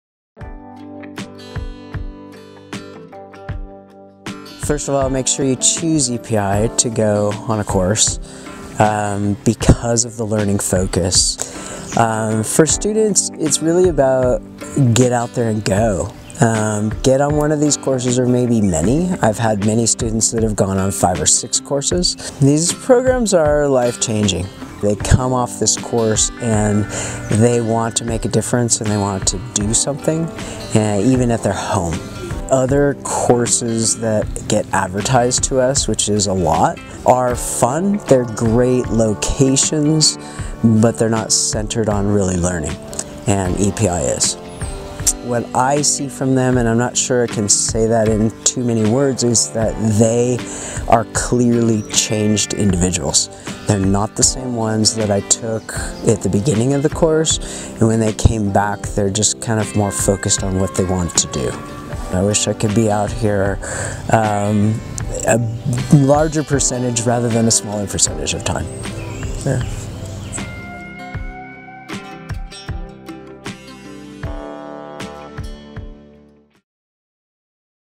Lessons from EPI's Most Experienced Chaperone!
EPI All-Star Chaperone, Tim Newman, sat down with us in the Greater Yellowstone Ecosystem to explain why he has chosen to be an EPI chaperone 26 times... and counting!